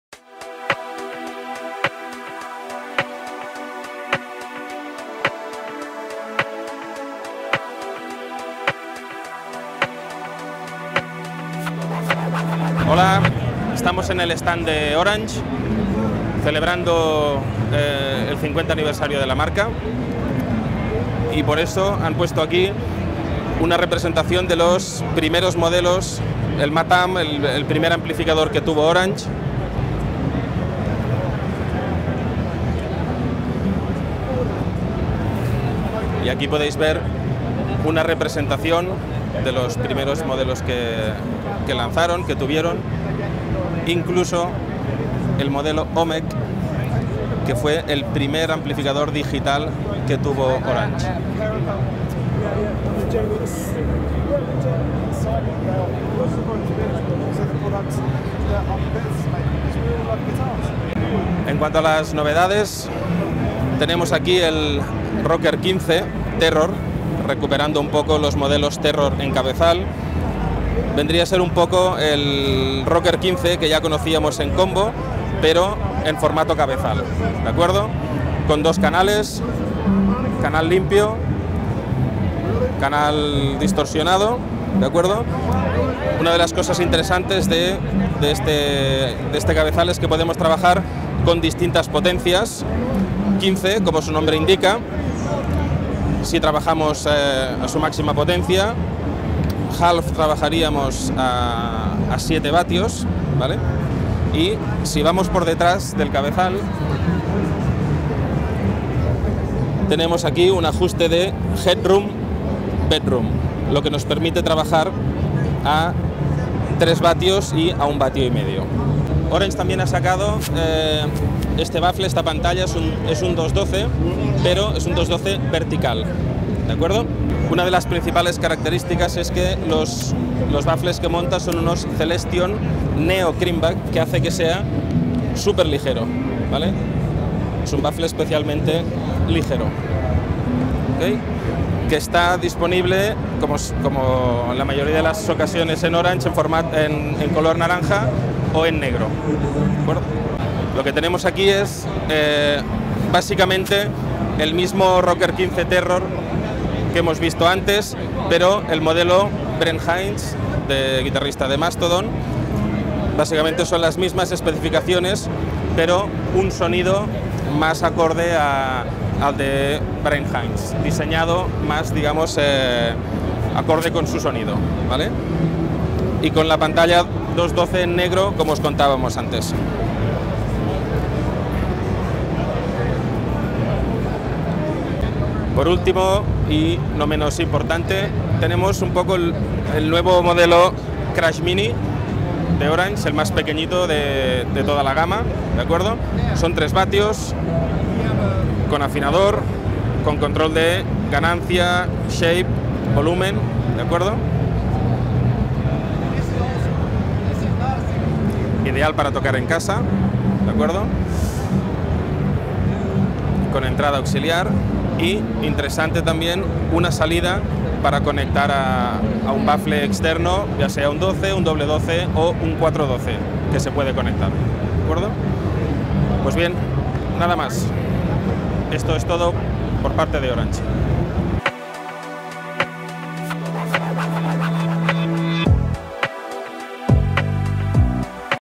NAMM2018 | ORANGE presenta sus nuevos productos!
ORANGE Amps para sus 50 años presenta nuevos productos durante el NAMM Show 2018